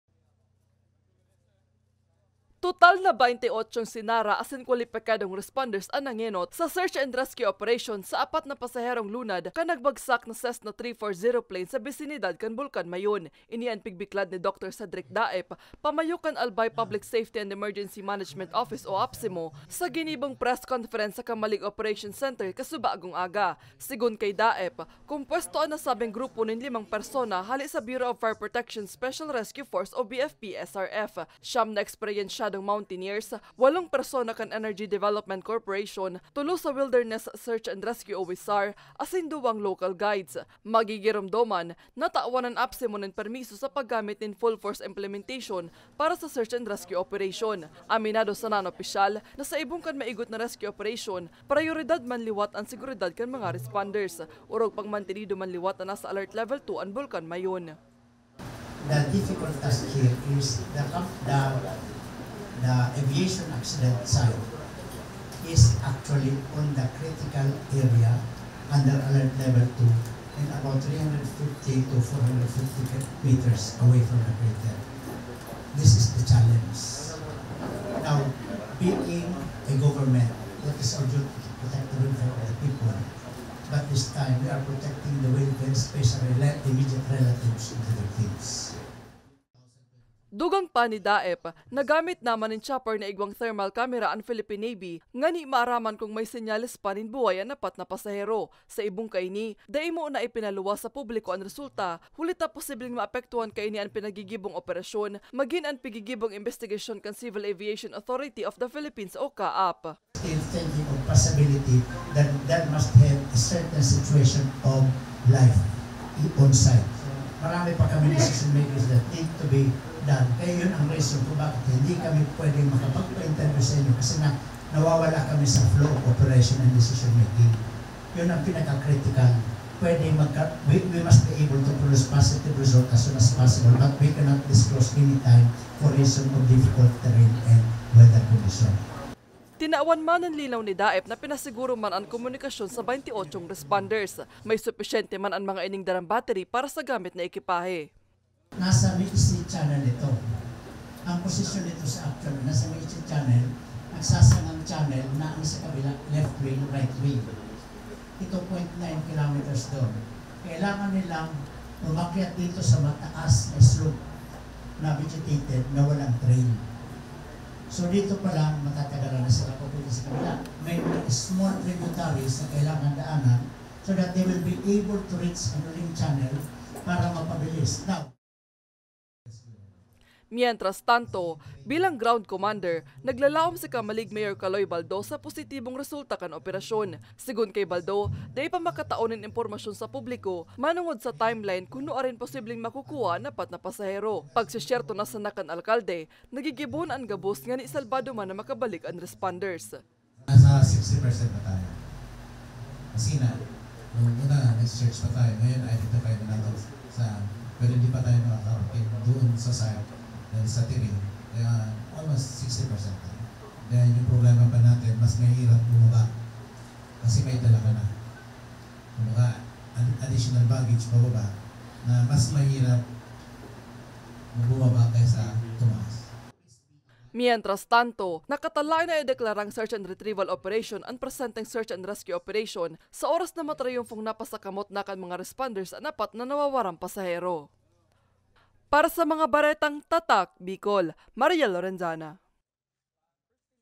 Search and Rescue Operation sa apat na lunad kan nagbagsak na CESSNA Plane, nagpapadagos
Search and Rescue Operation sa apat na lunad kan nagbagsak na CESSNA Plane, nagpapadagos; 28 responders, pasiring na sa crash site | via Marielle Lorenzana